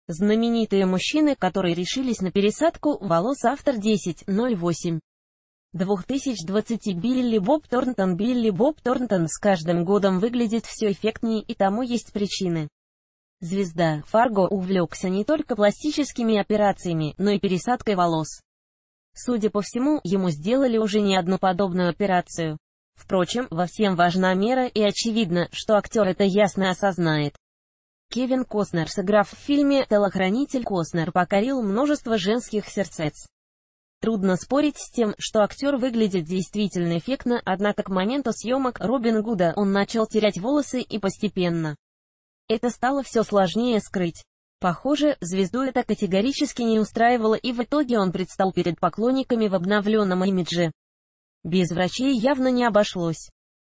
Знаменитые мужчины, которые решились на пересадку волос
В наши дни лысина не является недостатком для мужчин, и многие знаменитости, вроде Дуэйна Джонсона, Дмитрия Нагиева и Брюса Уиллиса, уже доказали это не себе. Впрочем, не все звезды готовы к такому брутальному образу, и всячески пытаются справиться с ситуацией.